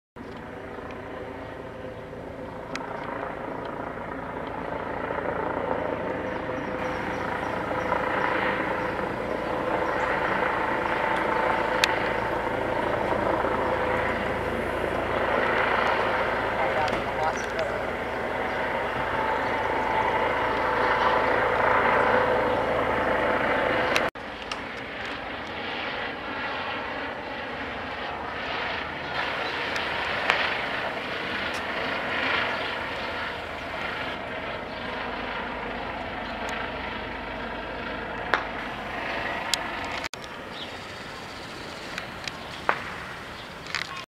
EC-135. STAT Medevac Helicopter from Youngstown Ohio.
June 18, 2013. STAT 8. N944ME. The Helicopter based at Youngstown Elser Metro Airport in April 2013 was N831ME. Corrections welcome.